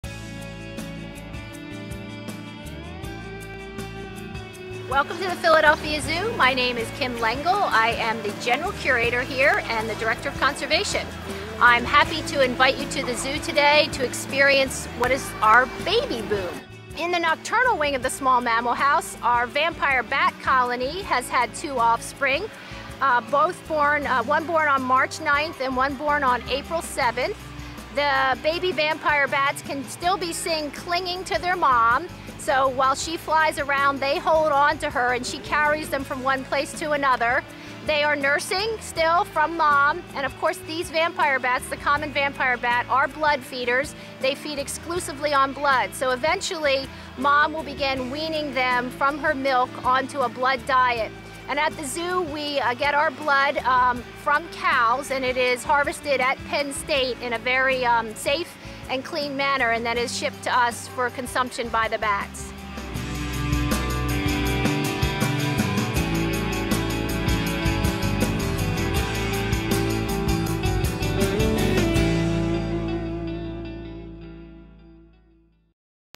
Philadelphia Zoo Baby Boom Series - Vampire Bat
The vampire bat's small but strong legs can reach speeds up to 4.9 miles per hour! The Philadelphia Zoo houses many of these skillful vampire bats including new babies born on March 9th and April 7th, come check it out!